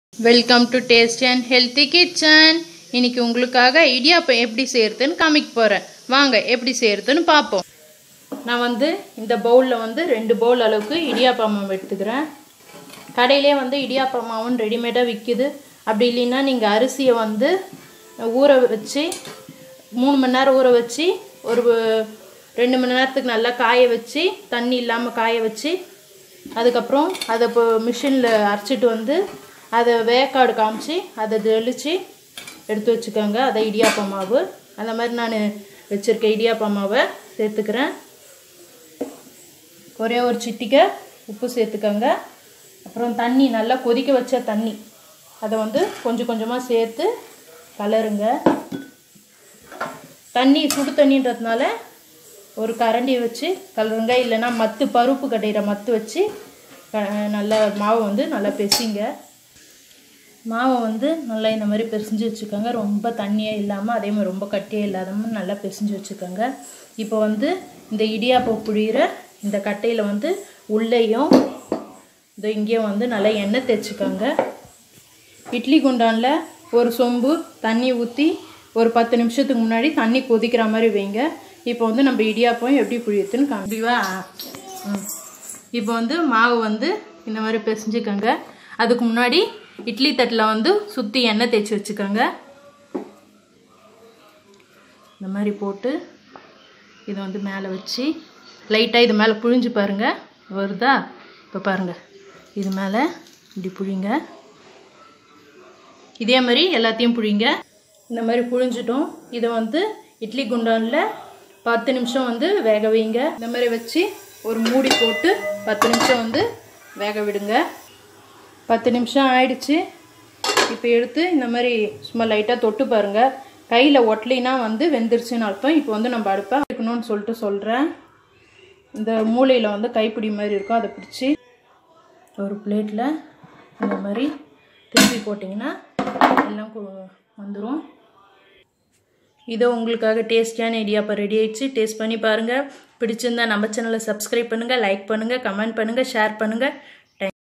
How to make ldiyappam in tamil/இடியாப்பம்/rice ldiyappam/simple break fast recipe
இடியாப்பம்,ldiyappam,rice ldiyappam,simple breakfast recipe

INGREDIENTS

Roasted rice flour 2cups
Boiling water few
oil few
salt few